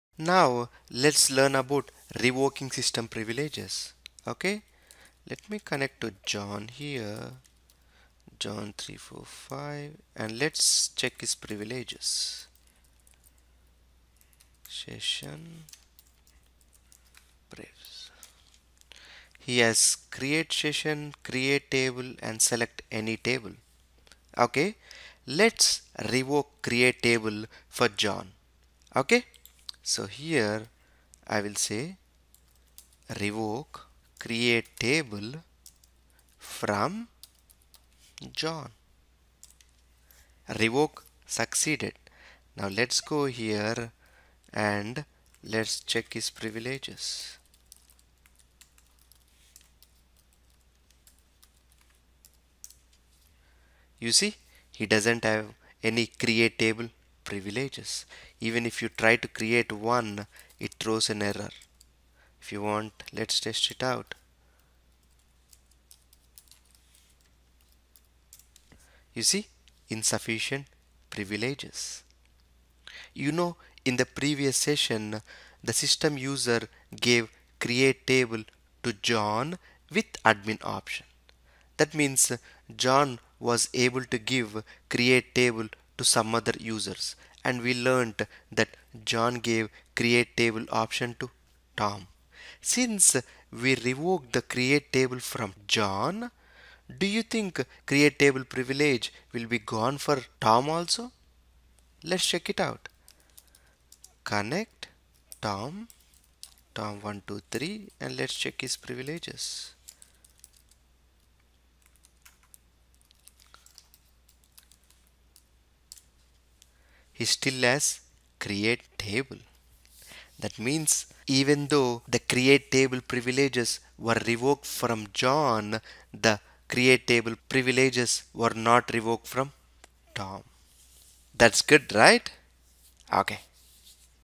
070 Oracle DBA Complete Tutorial - Revoking System Privileges
This video discusses revoking system privileges.